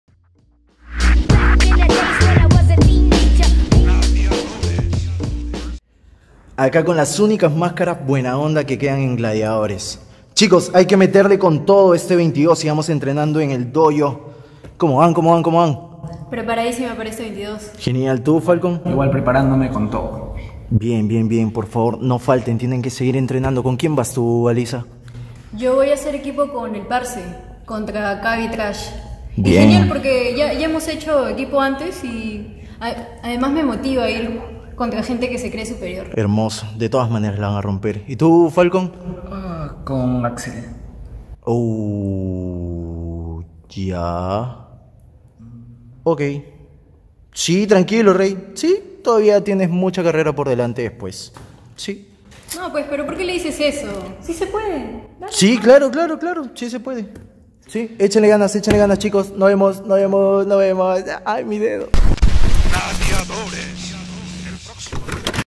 Los enmascarados "buena onda" del Danzak
Reptil nos comparte un video del final del entrenamiento junto a Alissa Webb y Falcon Kidd, y aprovechan para hablar de sus luchas de este sábado en Gladiadores 15: Respeto.

¿Todavía no tienes entrada? ¡Asegúrala de una vez que hasta el jueves hay descuentos solo por la web!